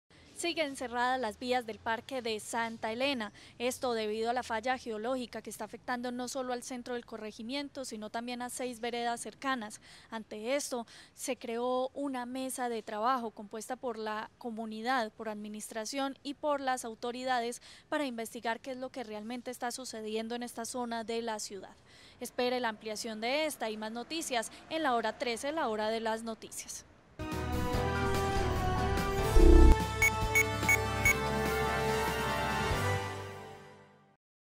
Tras cierre del parque en Santa Elena la comunidad y las autoridades conforman una mesa de trabajo
Se creó una mesa de trabajo compuesta por la comunidad,
 la administración  y las autoridades para investigar qué sucede en Santa Elena. Ampliación de esta y otras noticias en nuestra emisión de la 1: 00 p.m.